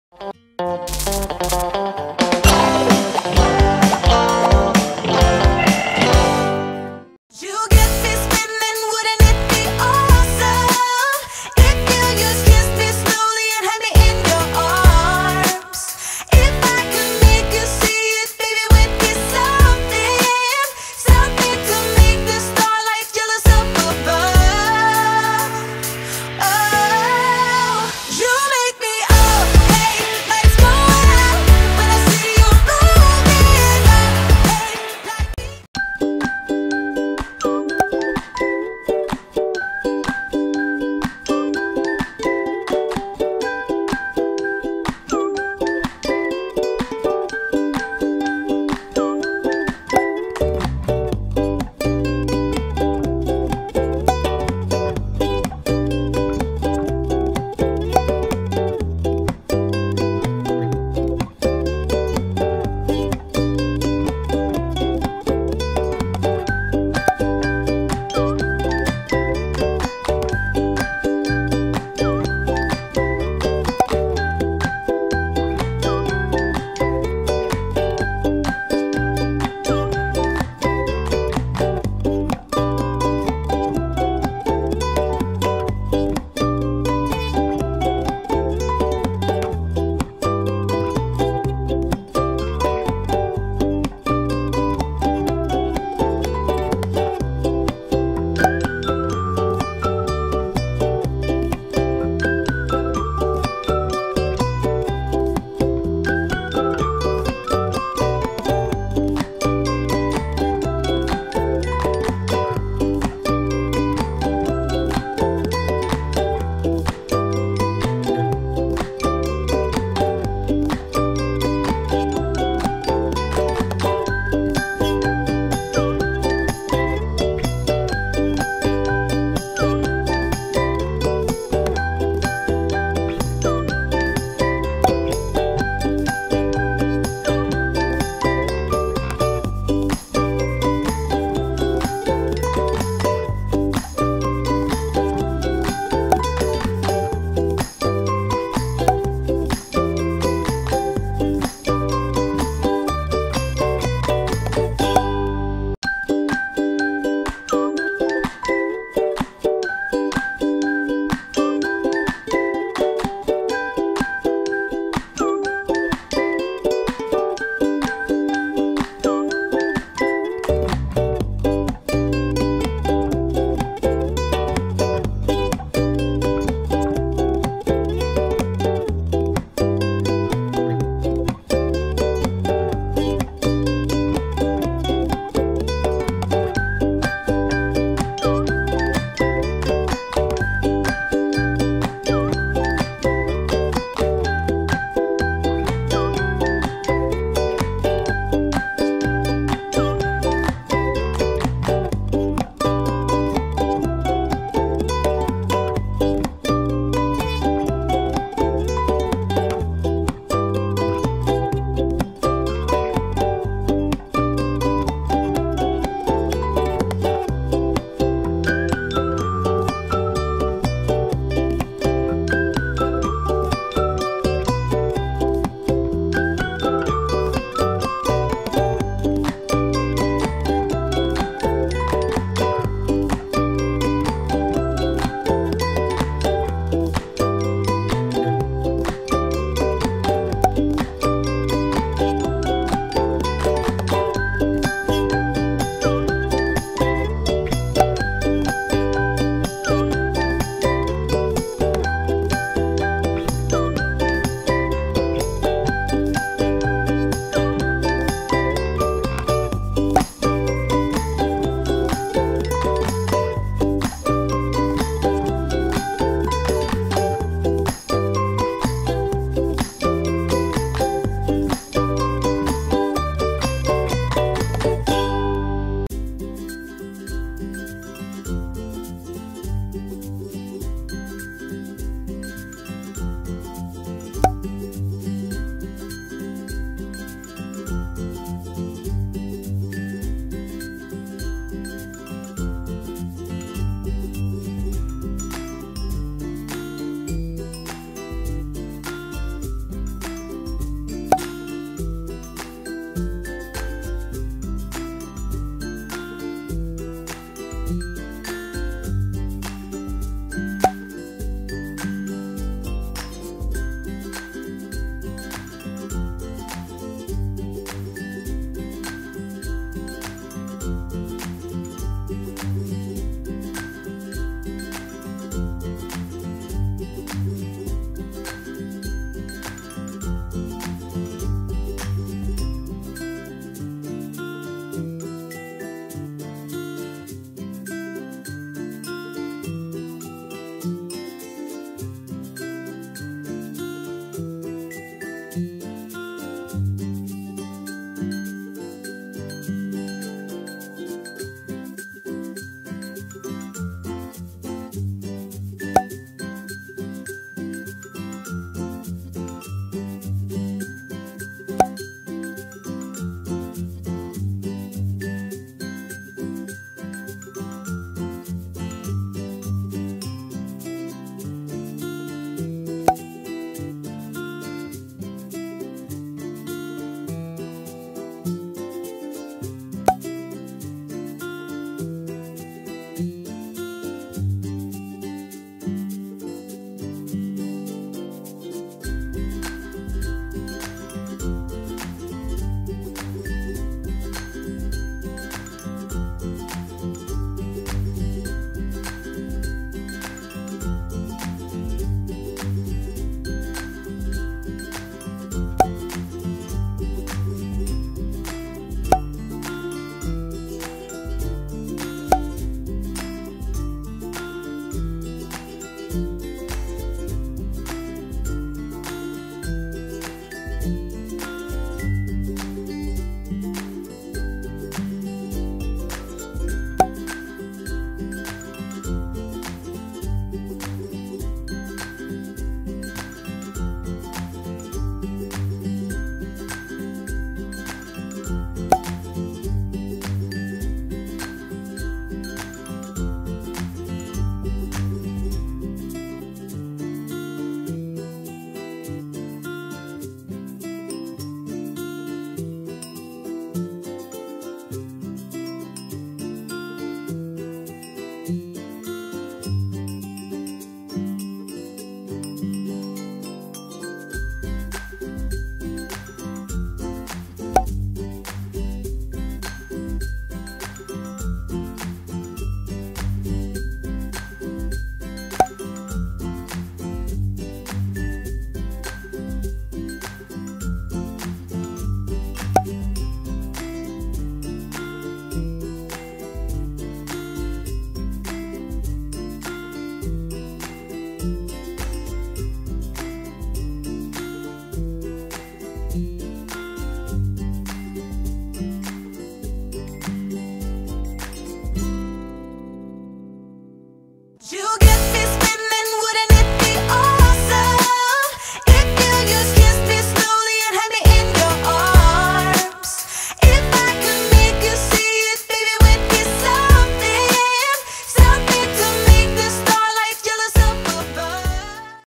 MAKE YOUR OWN VINTAGE SUITCASE WITH CARTON BOXES - Isa ❤️
To make this suitcase I used double-channel cardboard and advertising paper.

You know that I am in love with the romantic style and the pastel colors, so the deccoration of the suitcase could not be of any other style than the Shabbi Chic

The materials I have used are:

Double channel cardboard
Foam board
Compact cardboard of 1mm (it is the typical one used for cartonage)
Advertising Papers
White glue
cold silicone
Hot Silicone
Transfer glue
masking tape
Acrylic paint, in my case I used Chalk paint
Pieces of wood 0.5cm thick
One handle, 2 locks and 2 hinges
Adhesive vinyl paper for inside

If you want to send me a letter, drawing, photos etc ...

C.P: 45200 (Spain)
To Isa's World